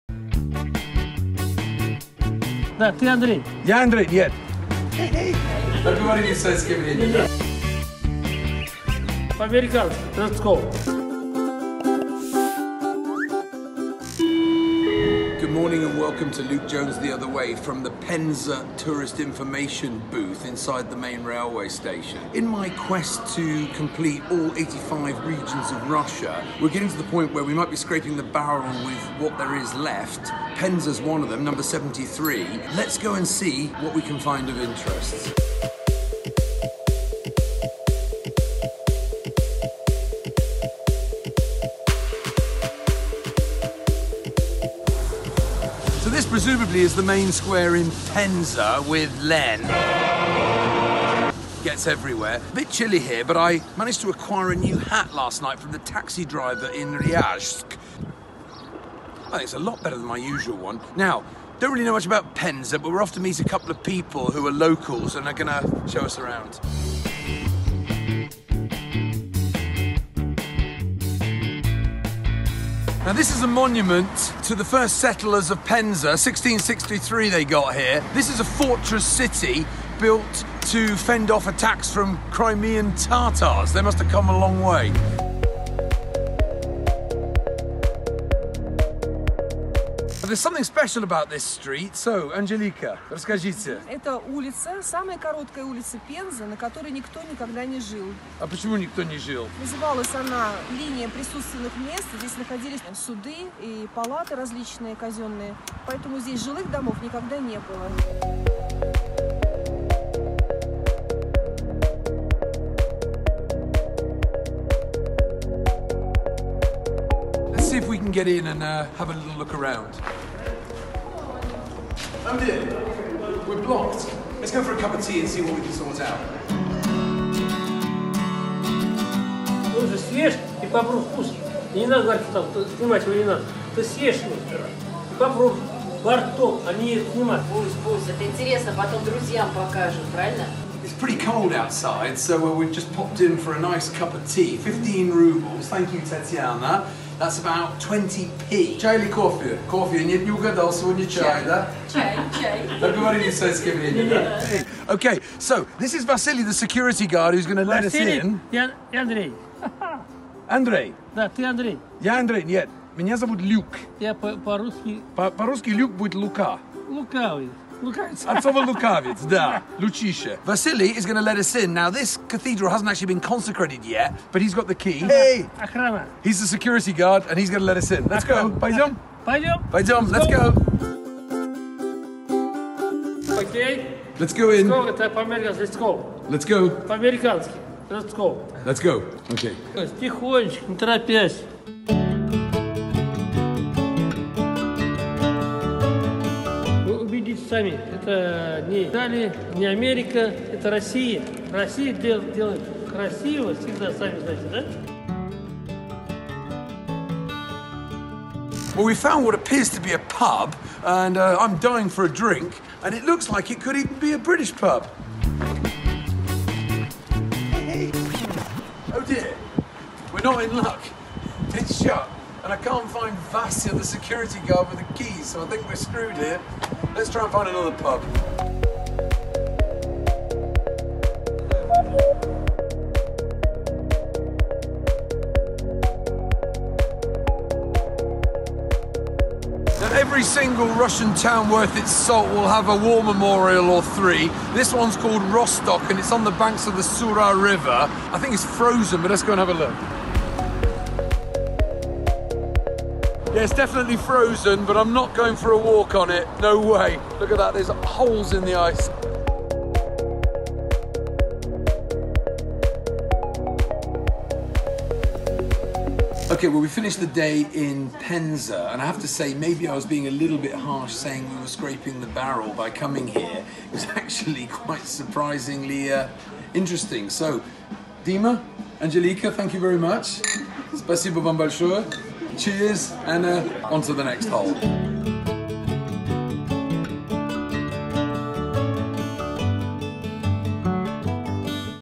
Penza - Russia! The Other Way
Pleasantly surprised by Penza. I initially imagined that Penza would be just another nothing town in the middle of nowhere, that I had to tick off in my quest to visit all 85 regions of Russia. But there was more to Penza than meets the eye - and a local character helped us on our way!